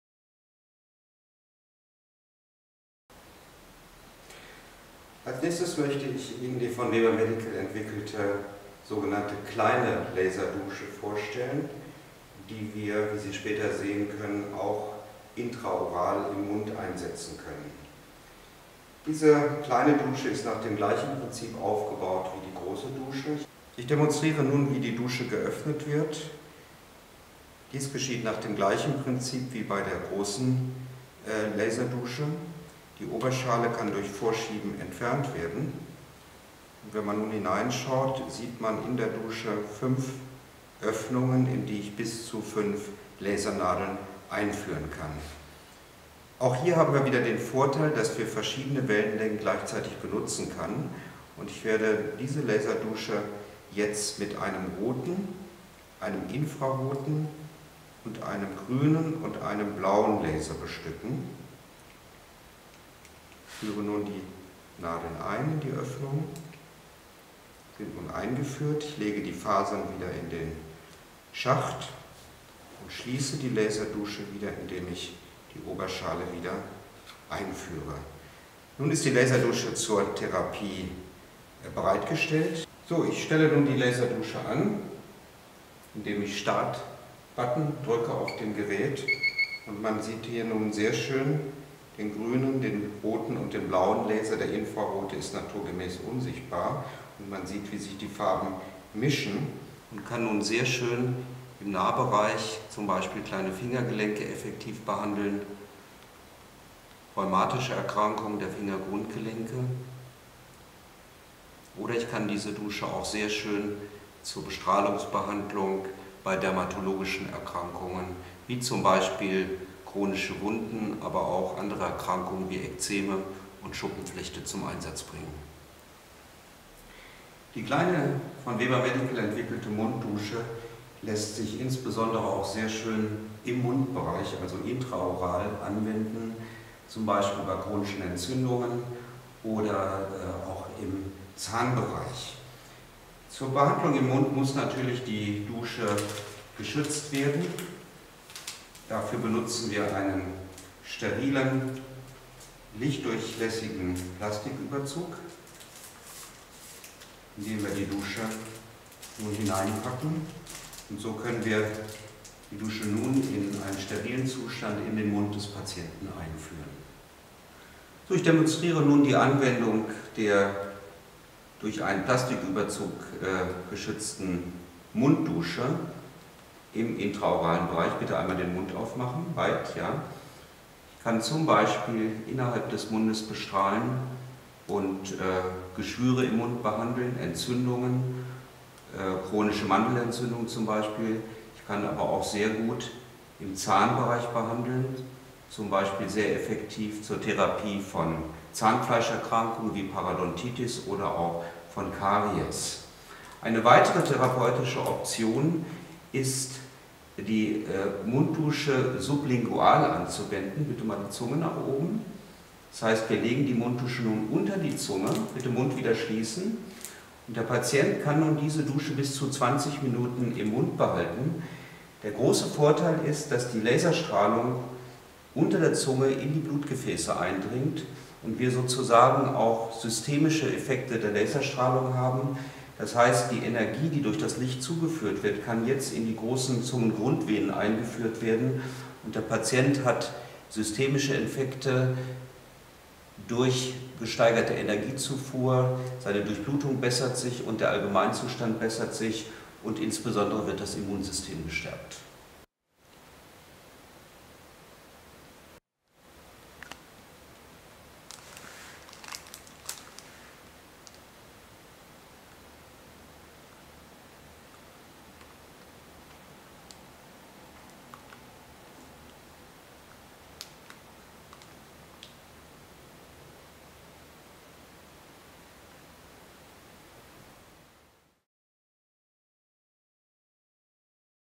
Externer und intra-oraler Einsatz der Weberneedle ® Munddusche
Einsatz des infraroten, roten, grünen und blauen Lasers des Weberneedle ® Compact zur Behandlung von kleinen Gelenken (z.B. bei Rheumapatienten), dermatologischen Erkrankungen (chron. Wunden, Ekzeme, Schuppenflechte, Akne), Entzündungen im Mund und Zahnbereich und Mandelentzündungen.
Demonstration der intra-oralen Behandlung zur Verbesserung der Durchblutung und Stärkung des Immunsystems durch systemische Effekte.